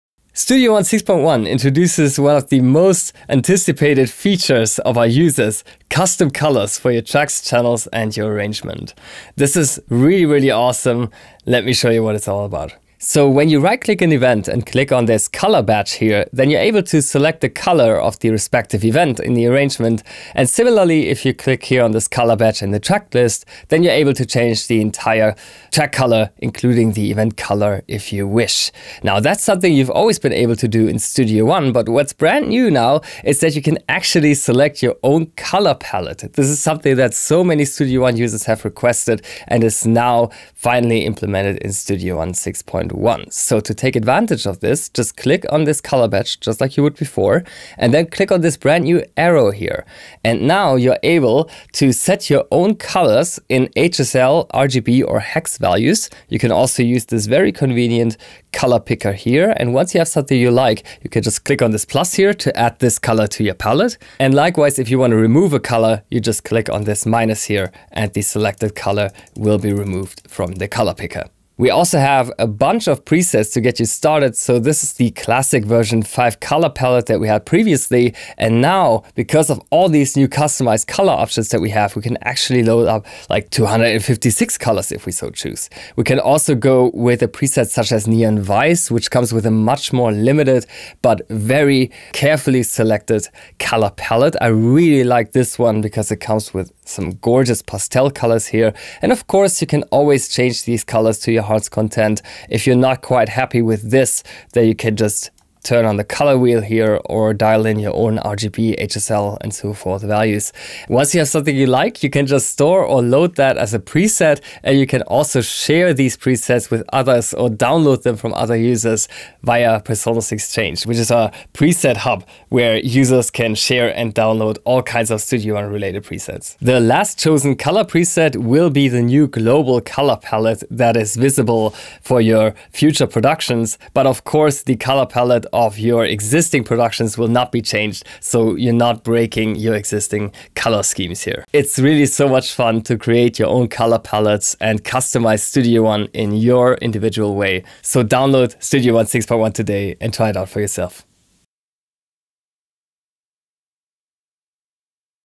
Studio One 6.1: Custom Colors
One of the top user-requested features is here! Gregor shows you how to change the color palette to match your needs and create your own custom presets.